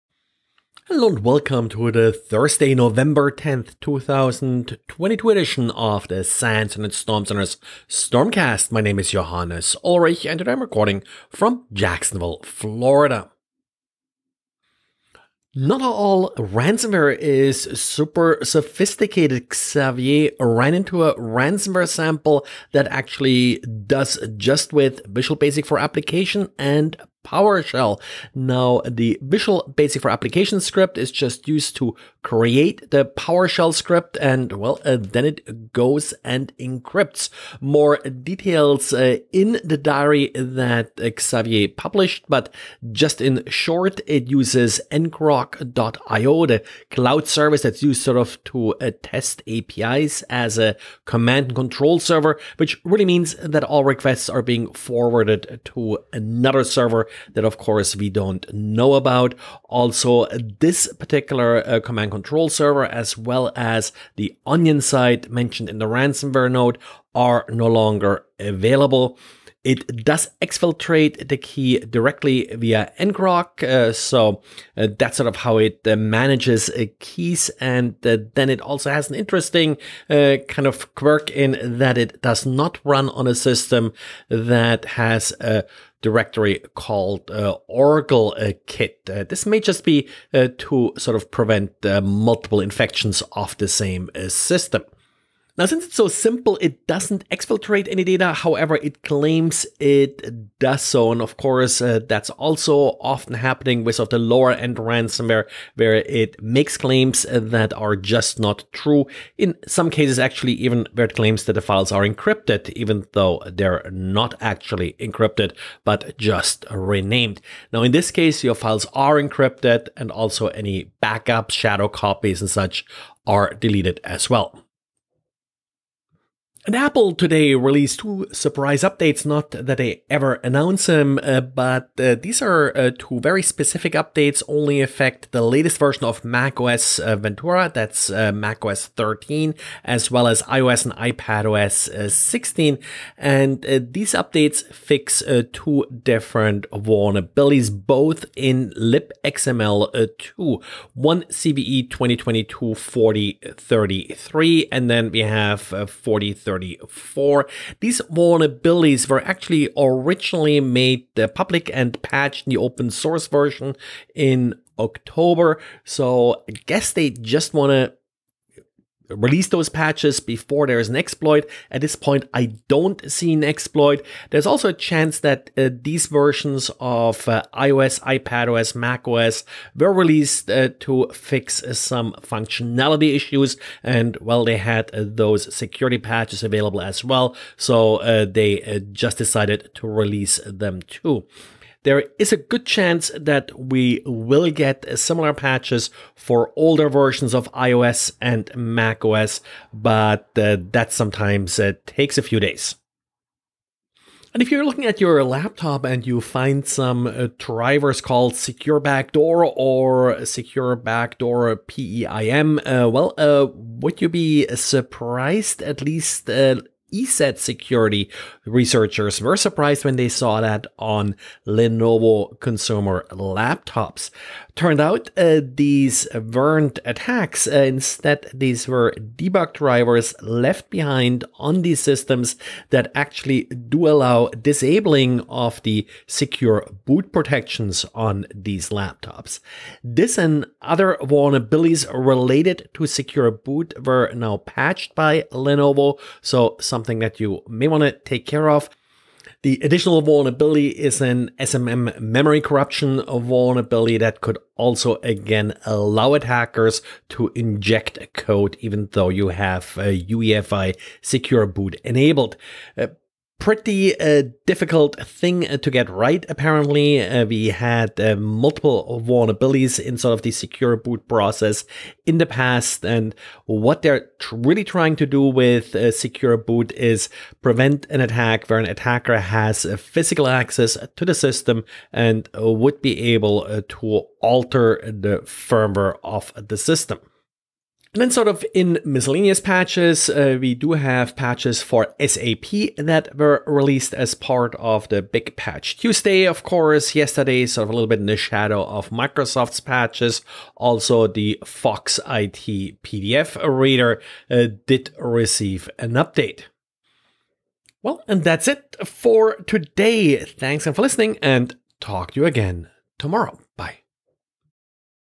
Network Security News Summary for Thursday November 10th, 2022
PS Ransomware; iOS/MacOS XML Patches; Lenovo UEFI Patch;